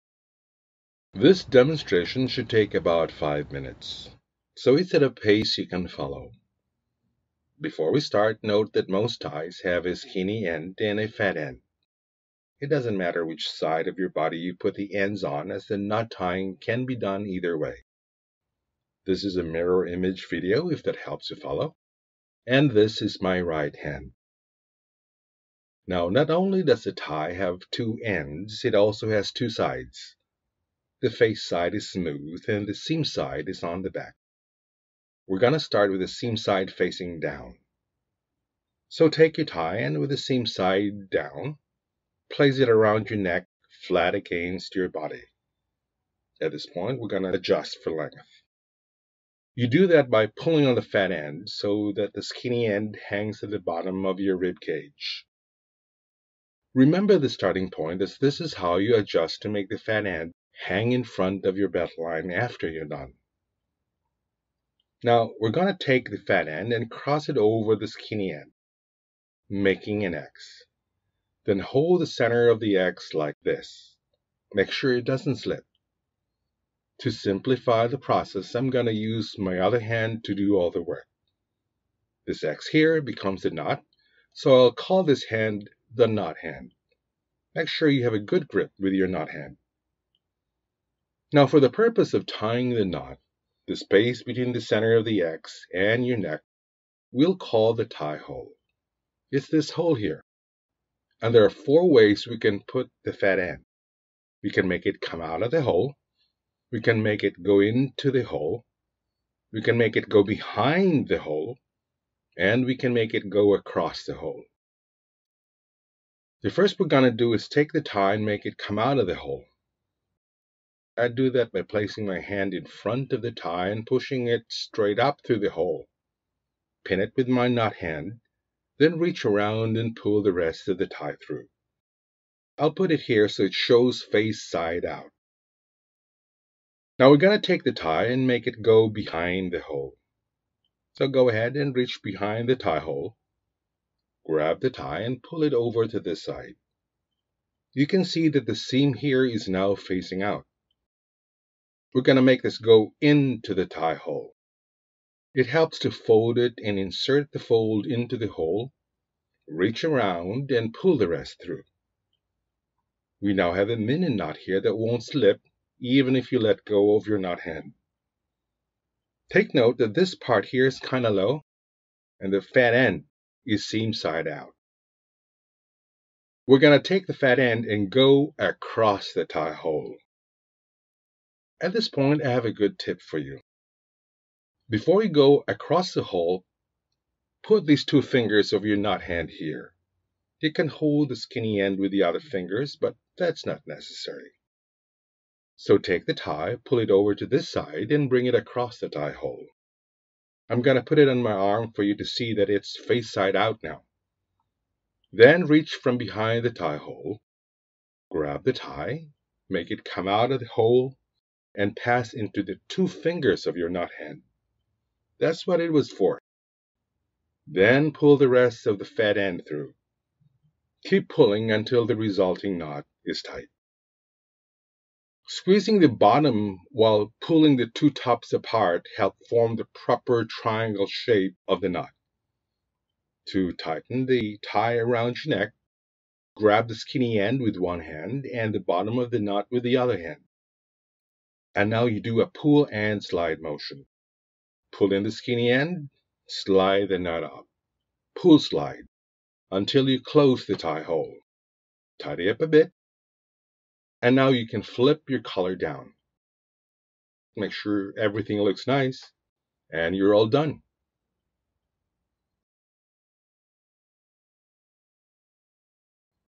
How To Tie A Tie: Full Windsor Knot (slowly, mirrored)
This demonstration should take about 5 minutes, so it's at a pace you can follow. Before we start, note that most ties have a skinny end and a fat end. It doesn't matter which side of your body you put the ends on as the knot tying can be done either way. This is a mirror-image video if that helps you follow, and this is my right hand.

Now, not only does the tie have 2 ends, it also has 2 sides. The face side is smooth and the seam side is on the back. We're going to start with the seam-side facing down. So take your tie and with the seam-side down, place it around your neck flat against your body. At this point, we're going to adjust for length. You do that by pulling on the fat end so that the skinny end hangs at the bottom of your rib cage. Remember this starting point as this is how you adjust to make the fat end hang in front of your belt line after you're done.

Now, we're going to take the fat end and cross it over the skinny end, making an X. Then hold the center of the X  like this, make sure it doesn't slip. To simplify the process, I'm going to use my other hand to do all the work. This X here becomes the knot so I'll call this hand the knot-hand. Make sure you have a good grip with your knot hand.

Now, for the purpose of tying the knot, the space between the center of the X and your neck we'll call the tie hole. It's this hole here, and there are 4 ways we can put the fat end. We can make it come out of the hole. We can make it go into the hole. We can make it go behind the hole, and we can make it go across the hole.

The first we're going to do is to take the tie and make it come out of the hole. I do that by placing my hand in front of the tie and pushing it straight up through the hole, pin it with my knot hand, then reach around and pull the rest of the tie through. I'll put it here so it shows face-side out.

Now we're going to take the tie and make it go behind the hole. So go ahead and reach behind the tie hole, grab the tie, and pull it over to this side. You can see that the seam here is now facing out. We're going to make this go into the tie hole. It helps to fold it and insert the fold into the hole, reach around and pull the rest through.

We now have a mini-knot here that won't slip even if you let-go of your knot hand. Take note that this part here is kind of low, and the fat end is seam-side out.

We're now going to take the fat end and go across the tie hole. At this point, I have a good tip for you. Before you go across the hole, put these two fingers of your knot hand here. You can hold the skinny end with the other fingers, but that's not necessary. So take the tie, pull it over to this side and bring it across the tie hole. I'm going to put it on my arm for you to see that it's face-side out now. Then reach from behind the tie hole, grab the tie, make it come out of the hole, and pass into the two fingers of your knot hand. That's what it was for. Then pull the rest of the fat end through. Keep pulling until the resulting knot is tight.

Squeezing the bottom while pulling the two tops apart help form the proper triangle shape of the knot. To tighten the tie around your neck, grab the skinny end with one hand and the bottom of the knot with the other hand. And now you do a "pull and slide" motion. Pull in the skinny end, slide the knot up - pull, slide - until you close the tie hole. Tidy up a bit, and now you can flip your collar down. Make sure everything looks nice, and you're all done.